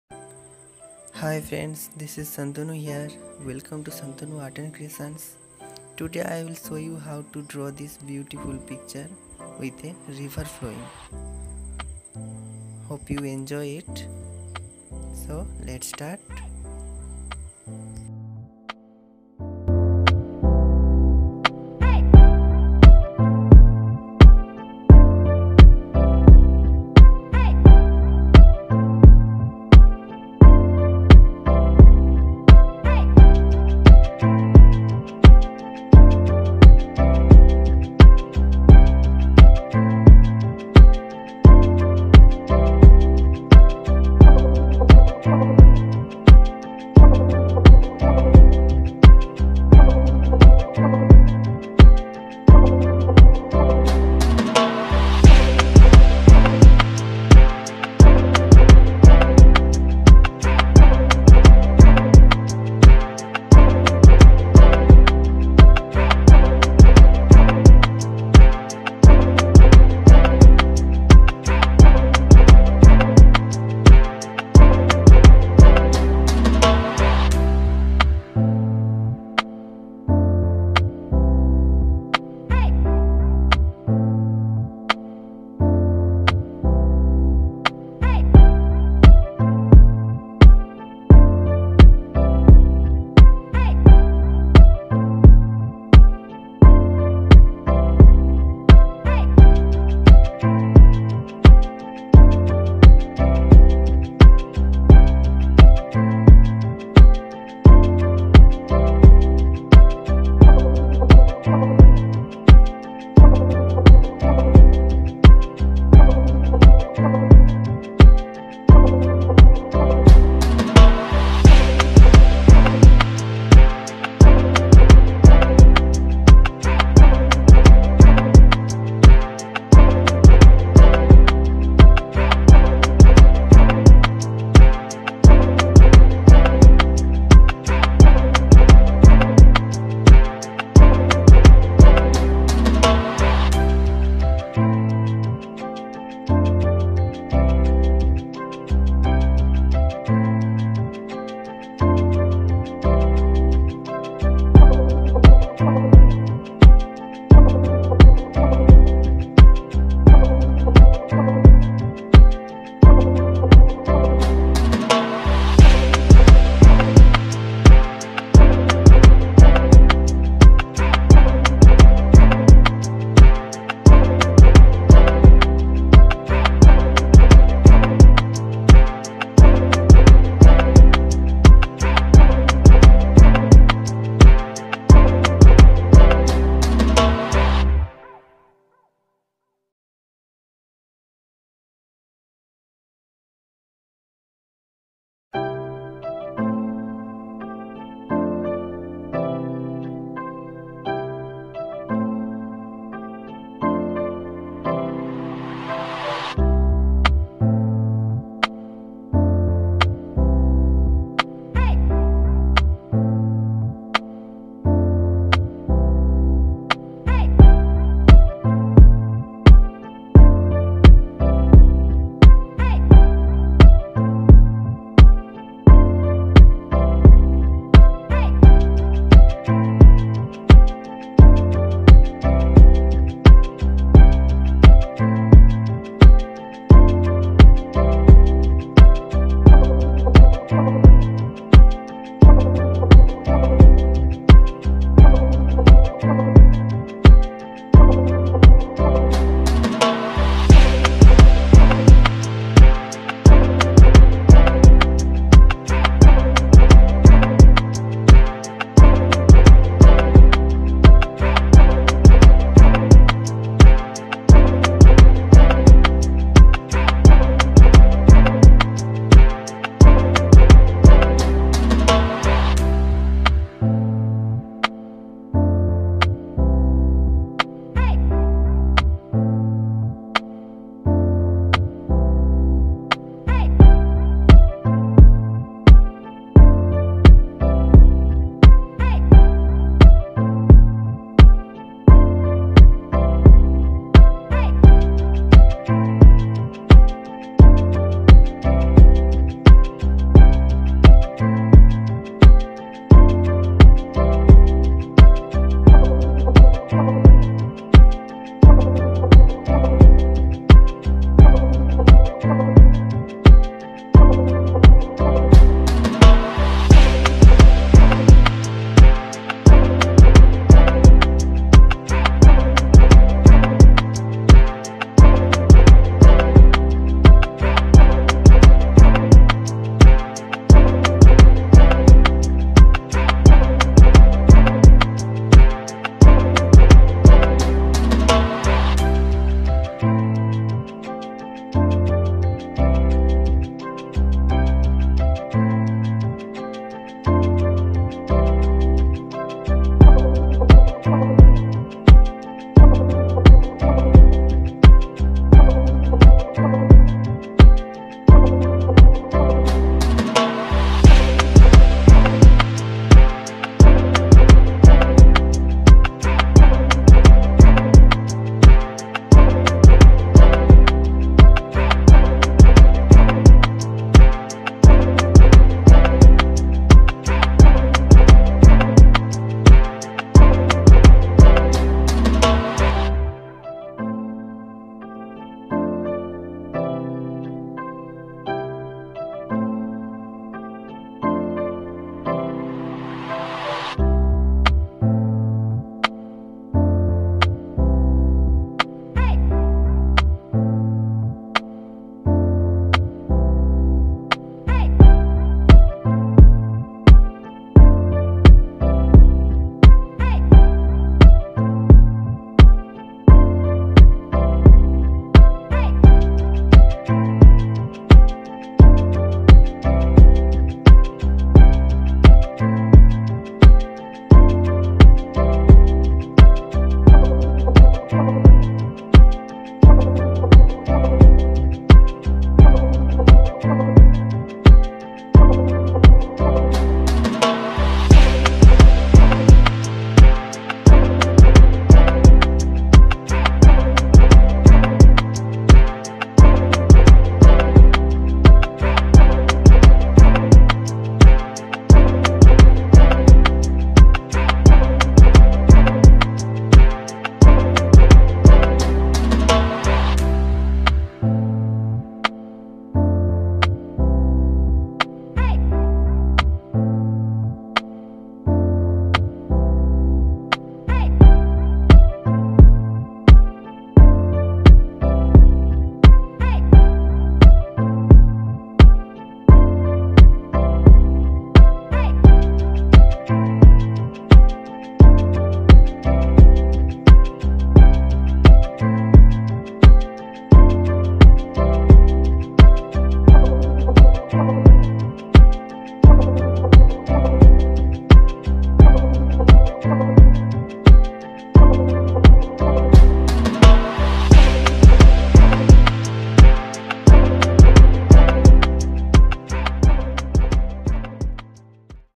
Autumn season scenery drawing with a river flowing
Autumn season scenery drawing with a river flowing is very simple but very beautiful with oil pastel colour..

Basic things..
 Art paper(rough side)
 Oil pastel colour 25/50 shades
 10b graphite pencil
 Masking tape